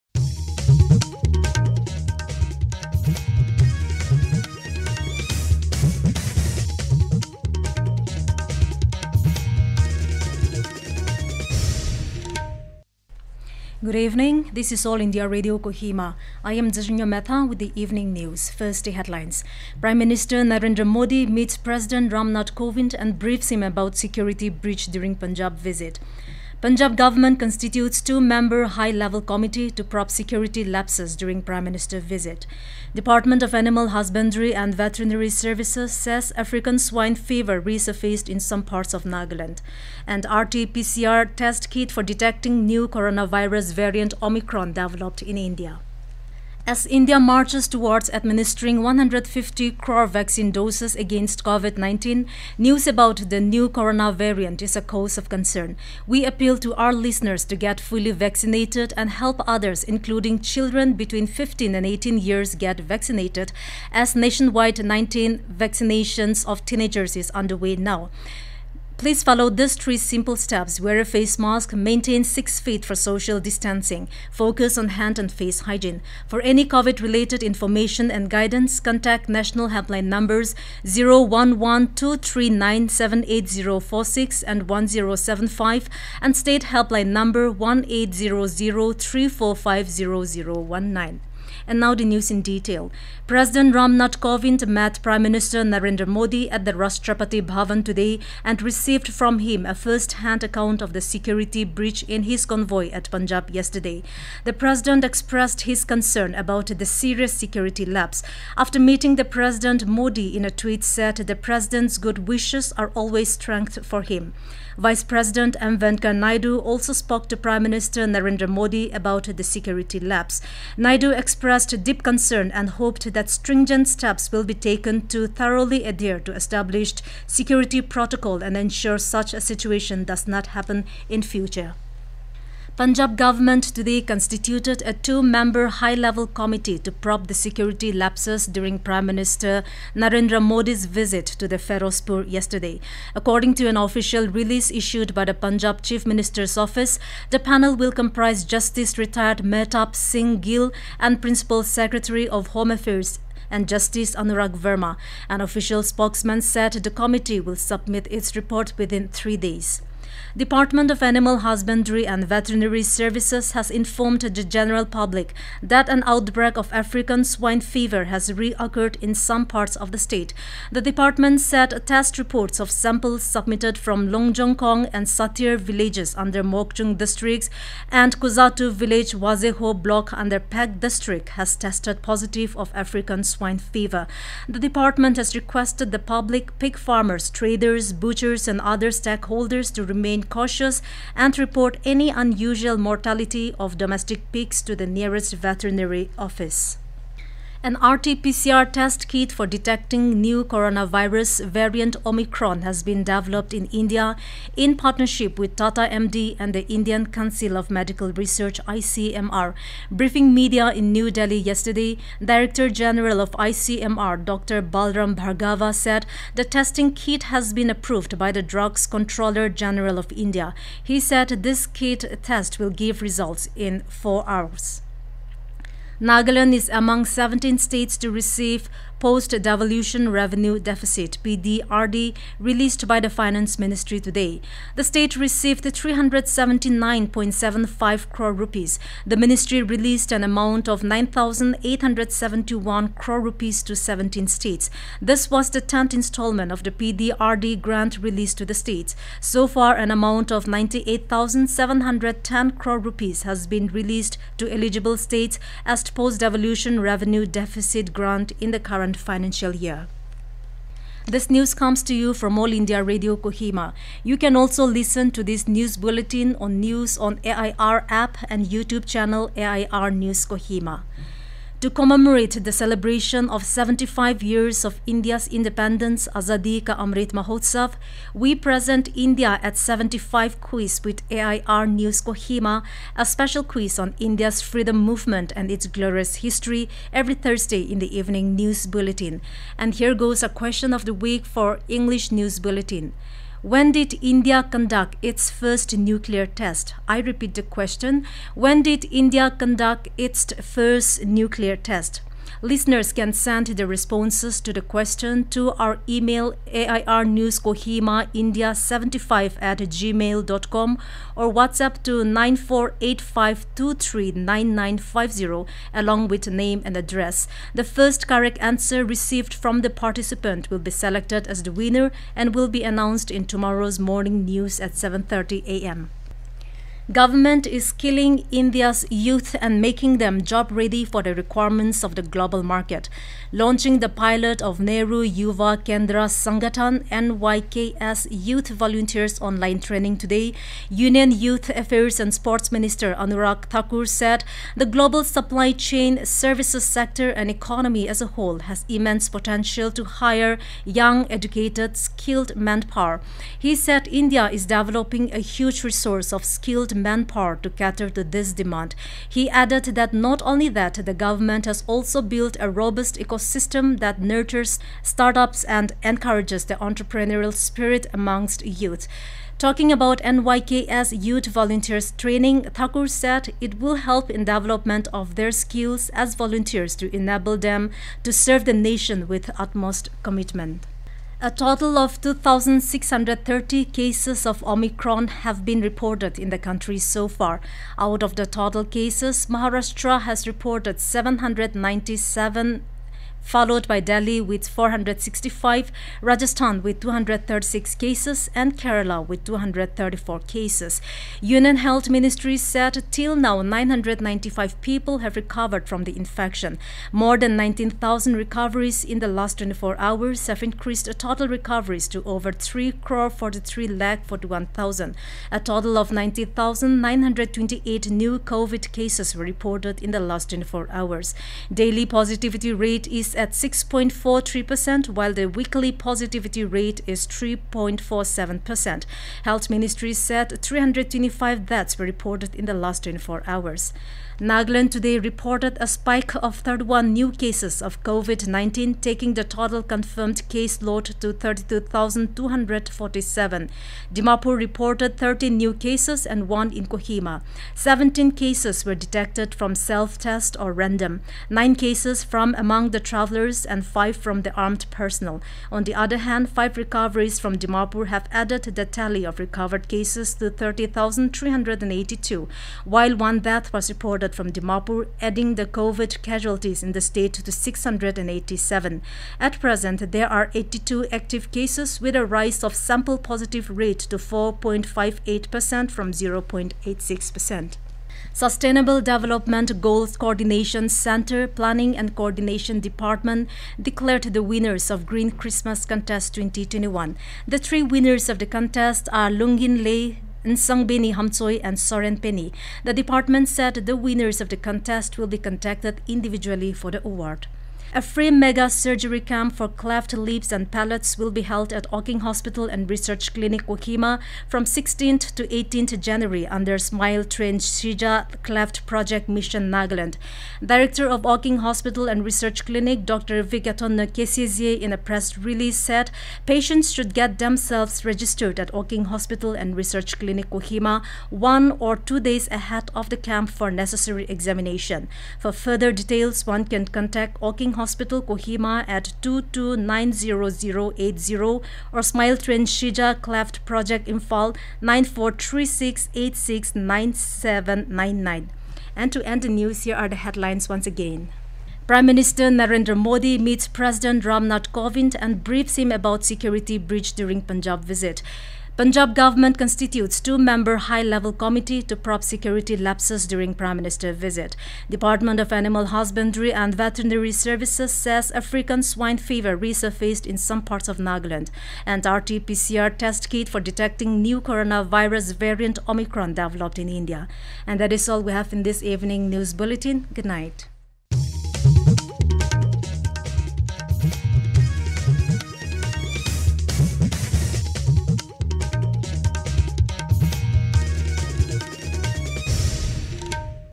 AIR News Kohima Evening English Bulletin on January 6, 2022
:::THE HEADLINES:::
1.  Prime Minister Narendra Modi meets President Ram Nath Kovind and briefs him about security breach during Punjab visit

2.  Punjab government constitutes two-member high-level committee to probe security lapses during Prime Minister visit

3.  Department of Animal Husbandry and Veterinary services says African Swine Fever resurfaced in some parts of Nagaland

4.  and RT-PCR test kit for detecting new coronavirus variant Omicron developed in India